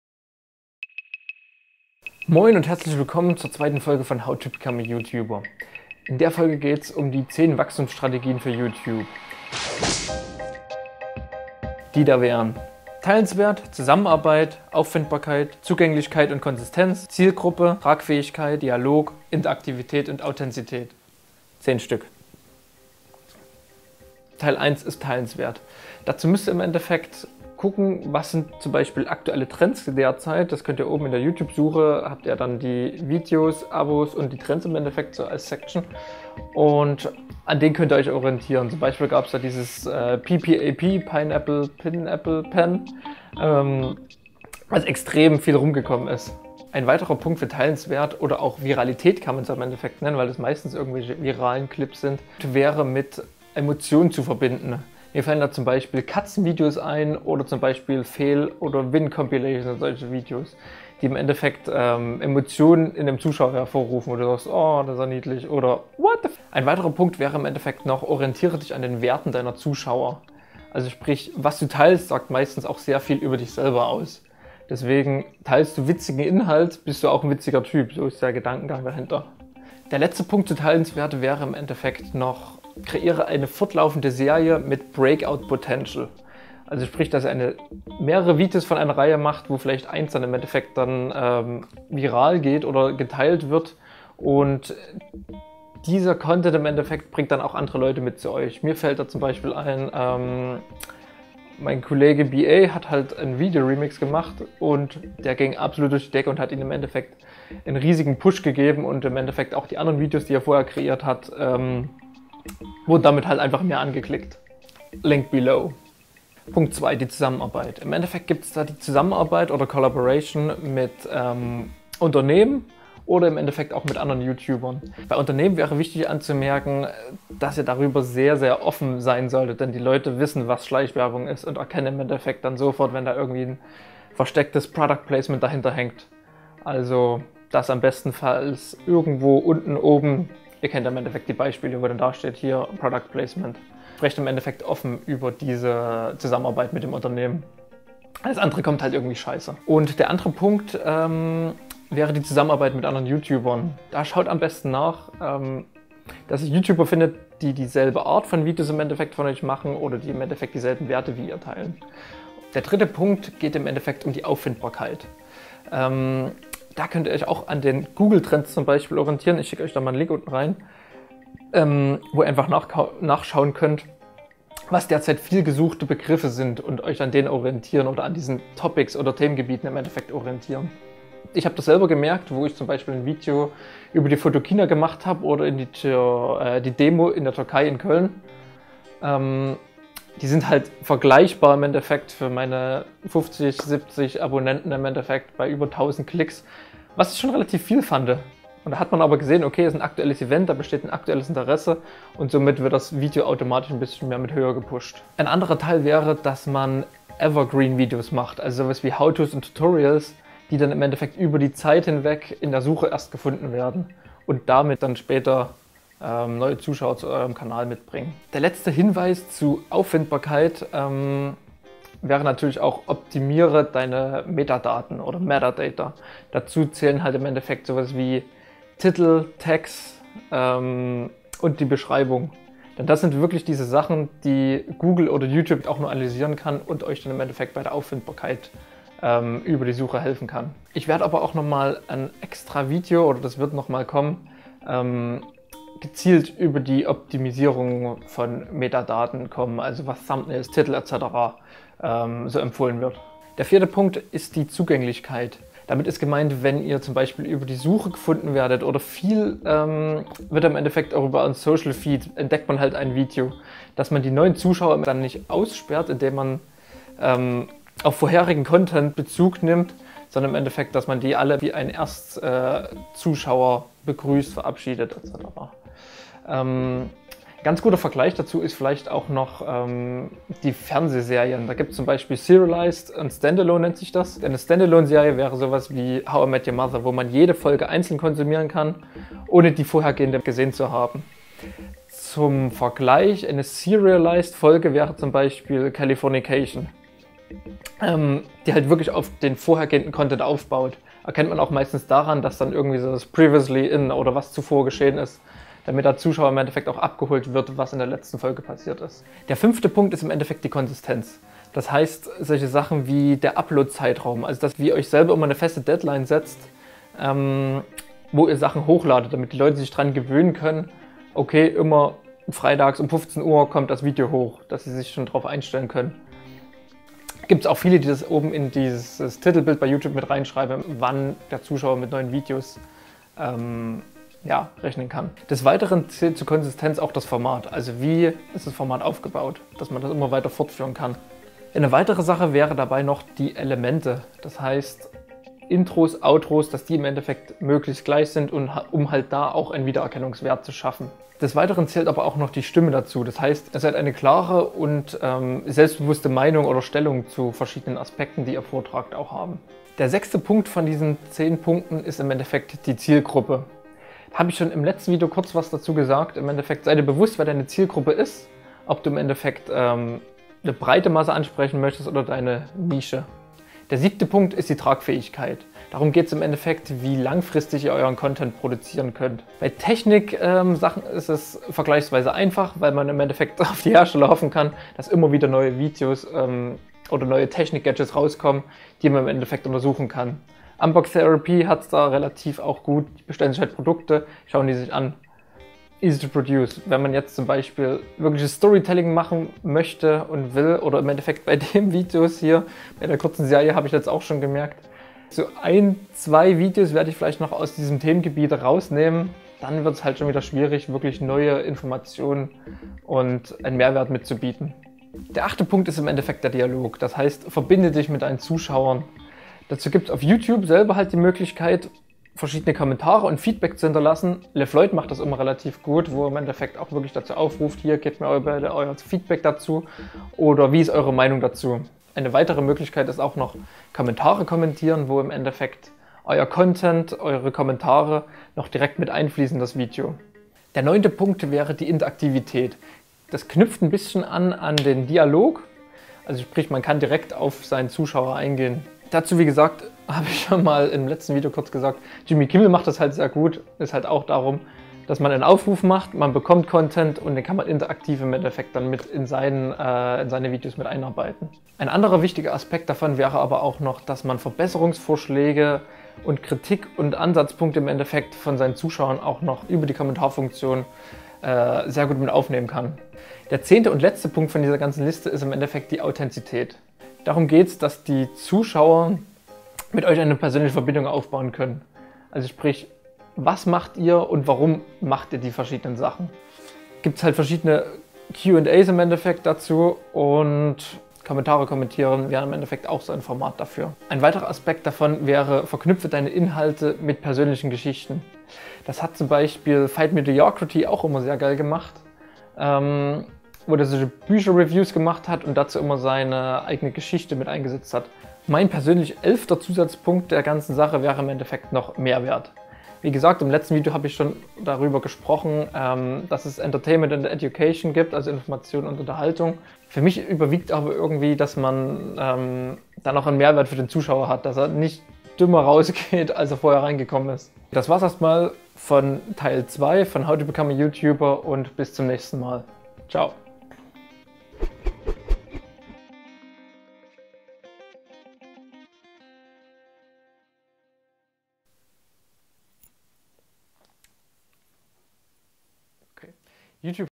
HOW TO become a YouTuber - Die 10 Wachstumsstrategien [Teil 2] (German Deutsch) The BlacBook
Im zweiten Teil von "How to become a YouTuber" geht es um die zehn Wachstumsstrategien für mehr Abos, Likes und Views.

1. Teilenswert
2. Zusammenarbeit
3. Auffindbarkeit
4. Zugänglichkeit
5. Konsistenz
6. Zielgruppe
7. Tragfähigkeit
8. Dialog
9. Interaktivität
10.Authenzität

Wenn ihr die Sachen selber nochmal nachlesen möchtet, findet ihr hier das "YouTube Playbook for Brands" aus welchem die Infos stammen:

"Im Endeffekt" ist mein "But I am"

[Shoot & Edit 13h]